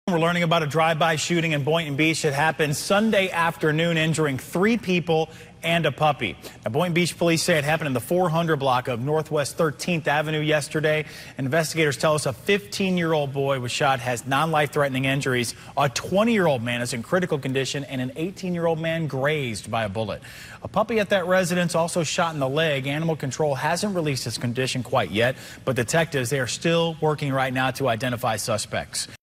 3 people, puppy shot in Boynton Beach
Boynton Beach police are investigating after three people were shot, including a 15-year-old boy, along with a puppy, on Sunday.

Police said the shooting happened just before 5 p.m. in the 400 block of Northwest 13th Avenue.